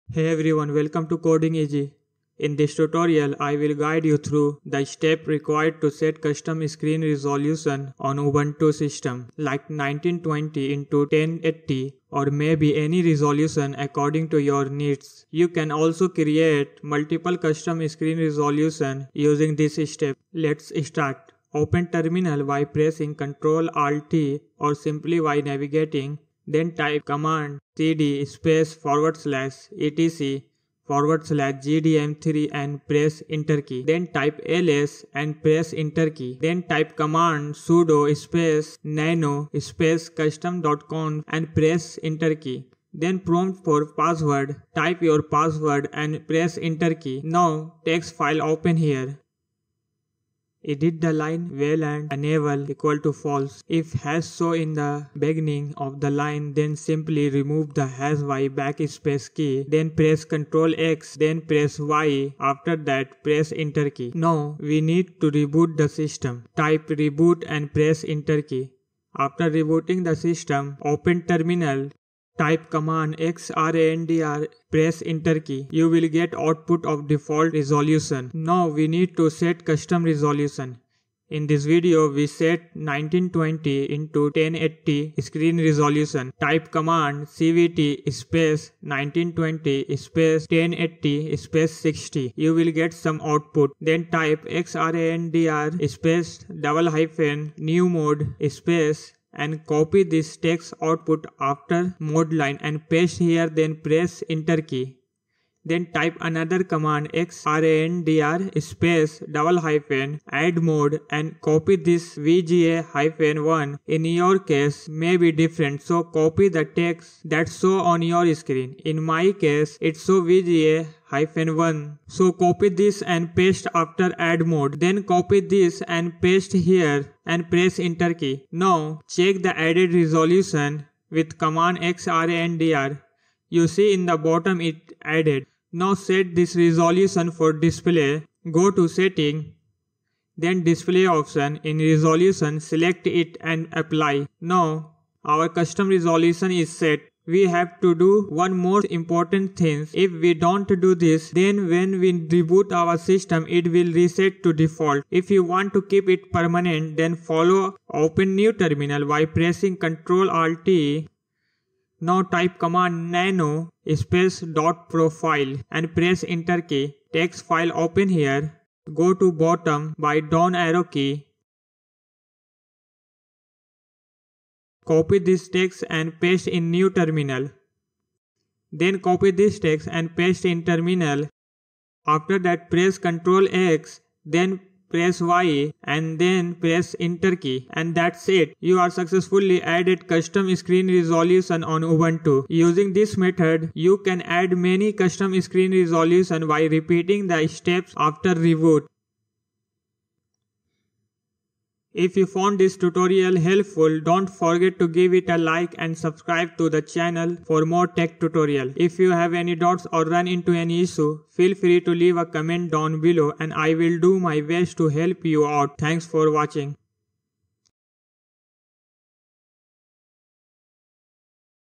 How to Set Custom Screen resolution in Ubuntu Using xrandr.1920x1080 resolution.
In this Video I'll guide you through the step by step process to add custom screen resolution in Ubuntu using XRANDR.
We can add multiple custom screen resolution using this method.
follow the video to set custom screen. resolution,

you can add custom supported screen resolution.
Commands used in the video (watch the video )

1➡️ xrandr

2➡️ cvt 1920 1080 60

 3➡️ xrandr --newmode "1920x1080_60.00"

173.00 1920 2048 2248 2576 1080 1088

1120-hsync +vsync

4➡️ xrandr --addmode VGA-1

"1920x1080_60.00"

5➡️ ls -a
6➡️ nano ./profile

For Better understand watch Video.